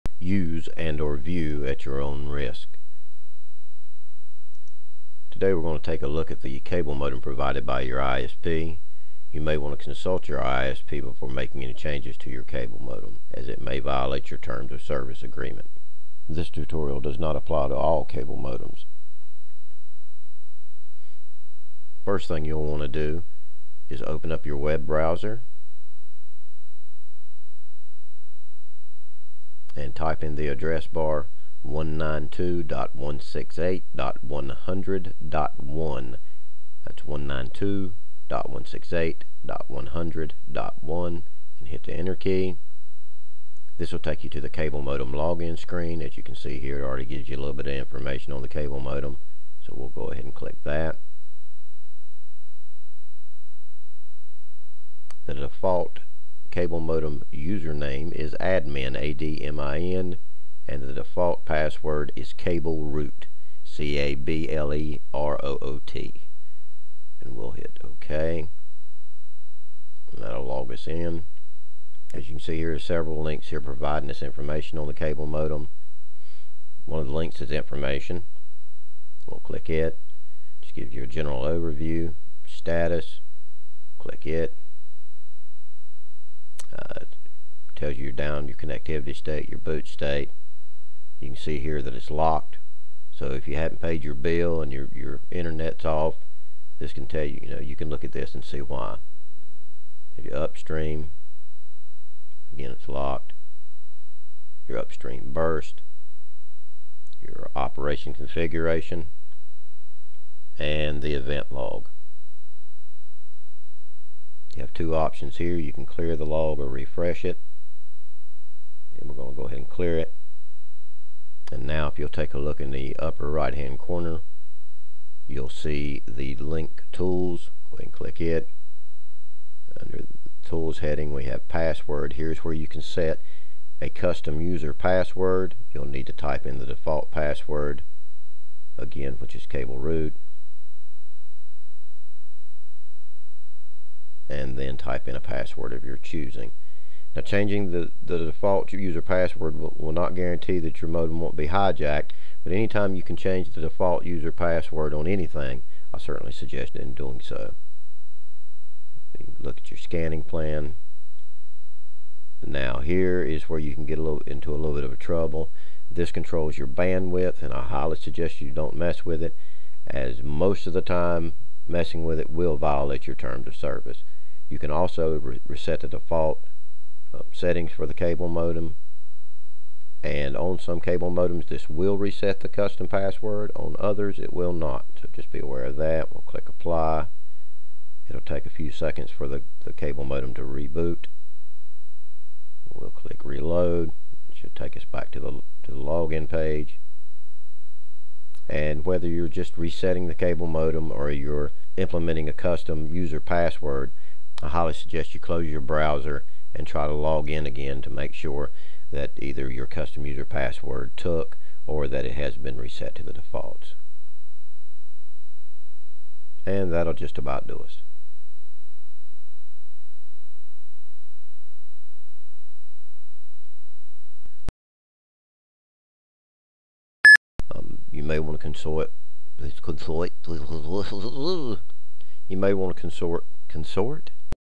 ISP Cable Modem : Login, Setting User Password & Restoring Defaults
A How-to on accessing, setting a custom password and restoring default setting for most Internet Service Provider Modems.
This cable modem's address: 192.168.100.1
Username: admin
Password: cableroot

In this video I'll show you how I Login to a Cable Modem, set a custom User Password and restore the default Settings. This particular Cable Modem is manufactured by Ubee. Your Cable Modem may be different.

To login to the Modem :
Open a Web Browser and in the Address Bar type-in "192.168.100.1" - then press the Enter Key. That will bring-up the main Login Screen. Once there, click the Login Link
- type-in "admin" for the User Name and "cableroot" for the Password - then click the Enter Key. ("admin" & "cableroot" are the default) Once logged-in several Menu Items will appear. Some of those are Information, Status, Downstream, Upstream and more. Each  Link will, ofcourse, show the corresponding Information.

To create or change the Password :
Click the "Tools" Link in the upper right-hand corner - then click the "Password" Link. Here's where you enter a custom Password. In order to change the Password, it's necessary to enter the previous Password. That can be the default Password or the last
custom Password. So, do not destroy the current Password until the new Password is set. I, also, strongly recommend trying the new Password before disregarding the old Password. Once the old and new Passwords have been entered - click the "Enter" Button.

To restore the default Settings, for the entire Modem :
Click the "Tools" Link in the upper right-hand corner - then click the "Reset Defaults" Link - select "yes" - then click the "Apply" Button.